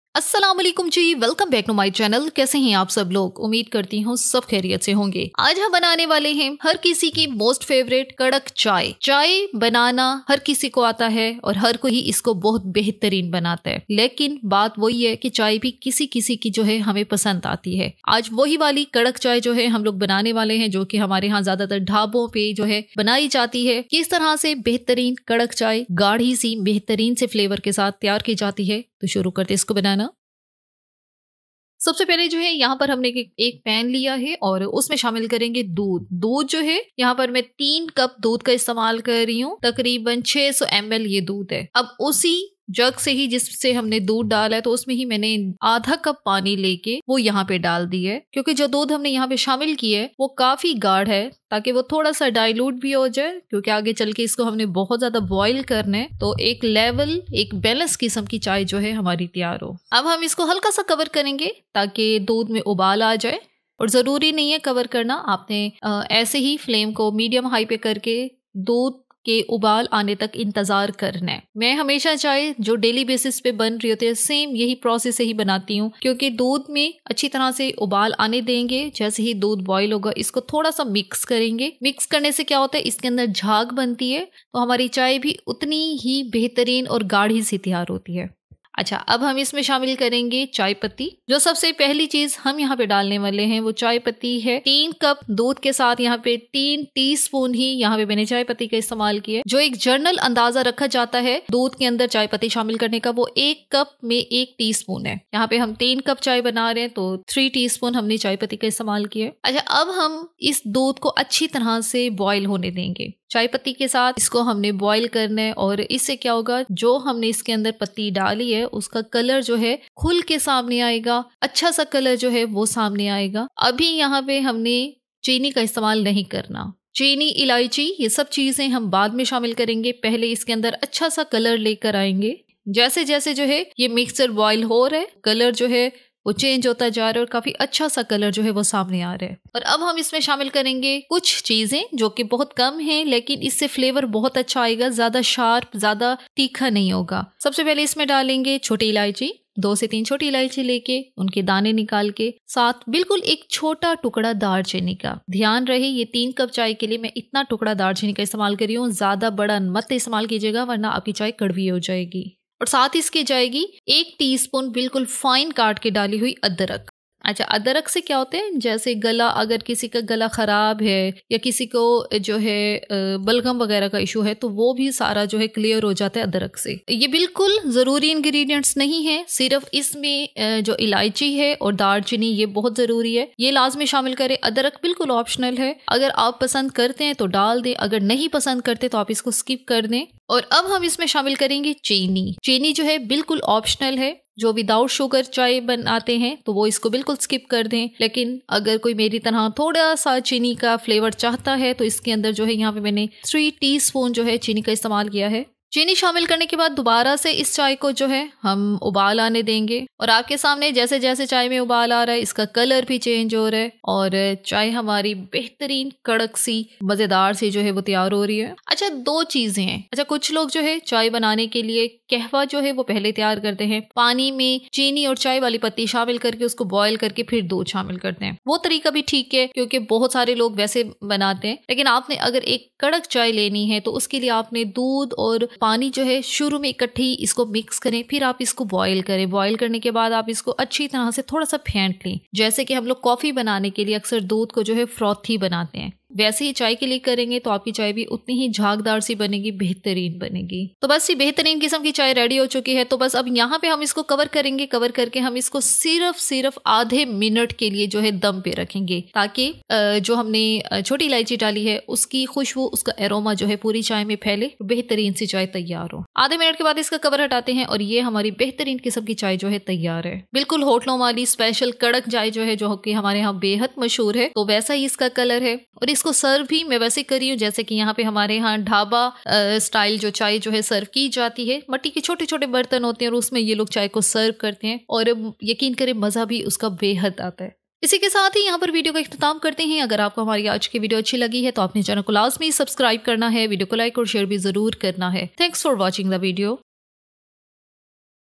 Most popular- Karak Chai Recipe by Samina Food Story
Karak chai is a popular spiced tea from the Indian subcontinent, especially loved in countries like India, Pakistan, and the Gulf states. It’s known for its strong flavor and creamy texture. Here’s a basic recipe:

Recipe Ingredients:
Milk : 3 cup or 600 ml
Water : 1/2 cup
Cover it for 30 seconds
Tea leaves / chai patti: 3 tsp
Cardamom : 3-4
Cinnamon: 1
Ginger chopped

Hope you are enjoying our recipes!